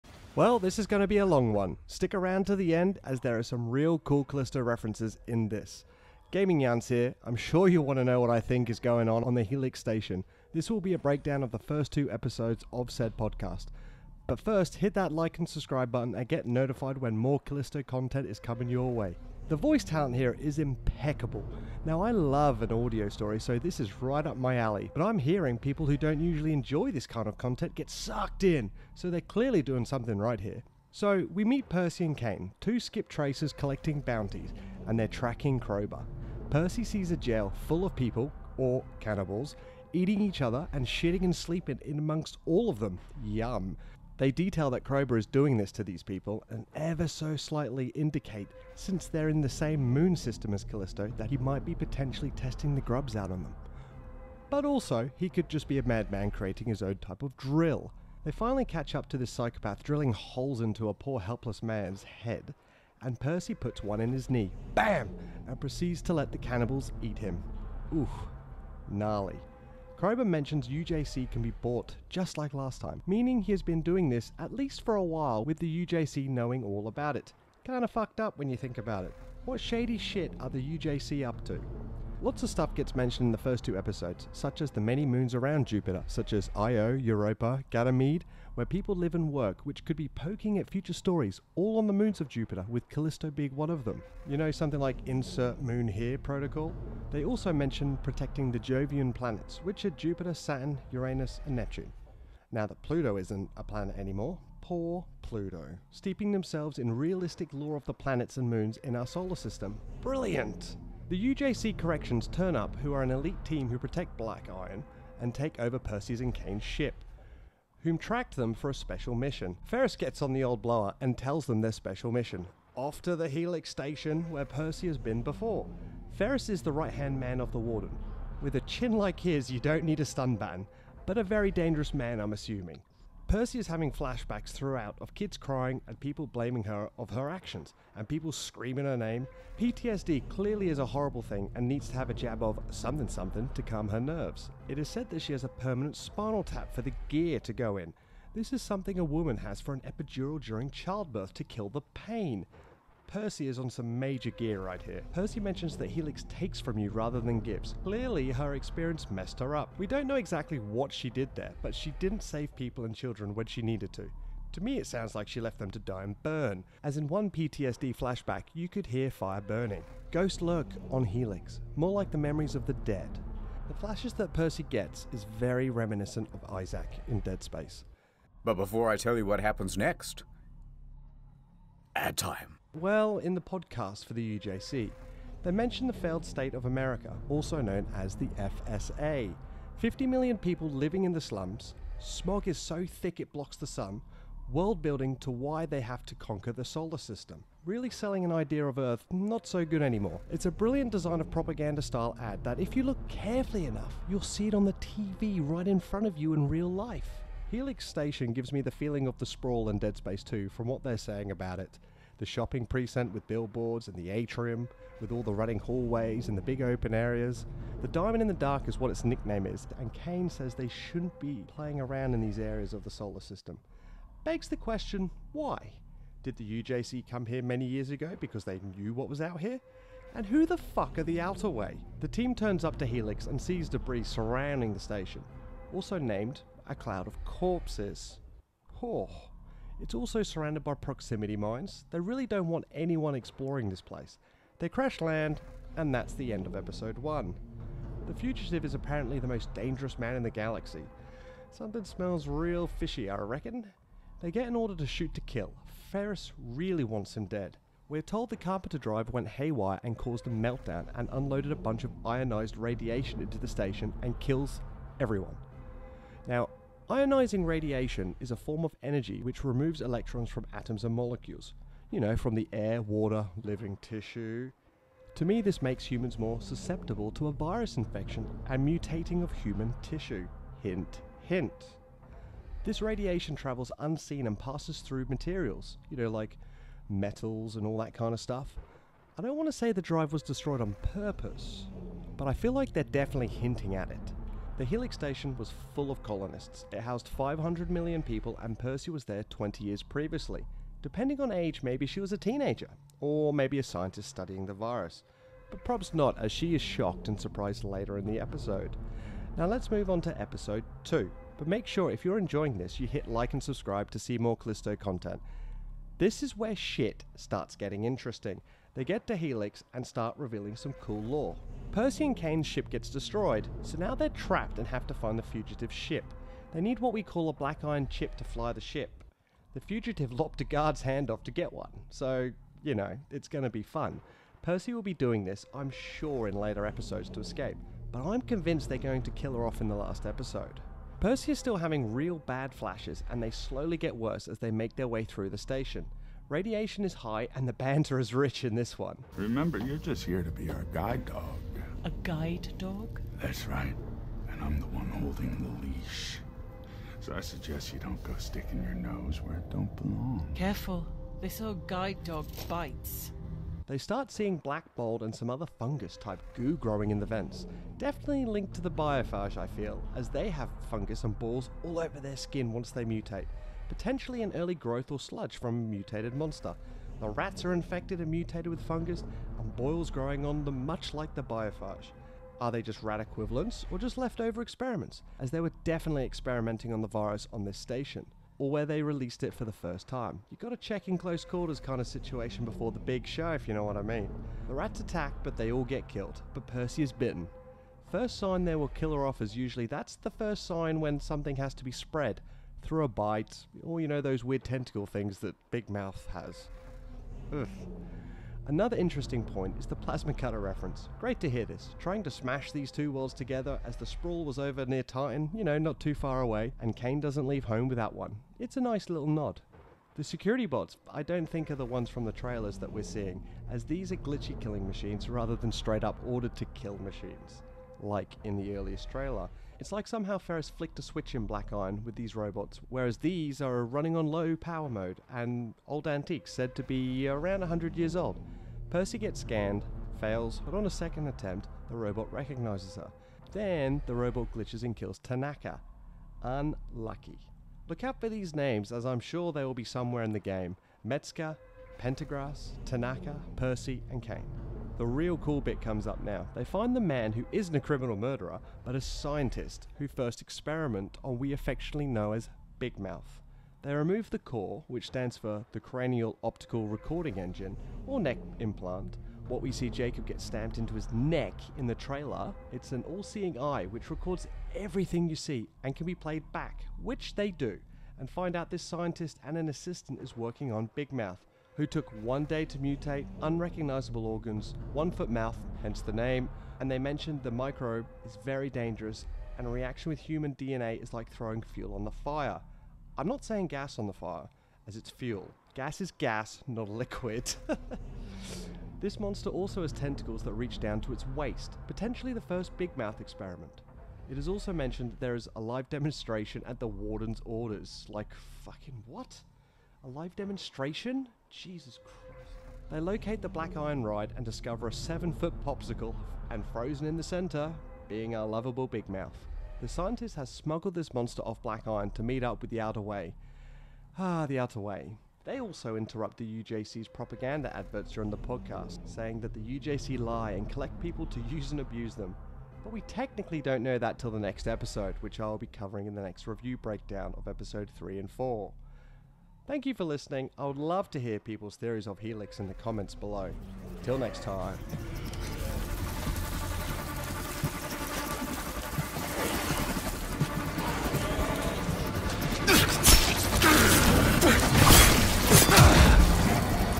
Helix Station EP1 & EP2 Breakdown
A discussion all about the Helix station podcast episode 1 and episode 2. Secrets and easter eggs revelead.  Callisto Protocol Lore has been discovered and how the two link up together.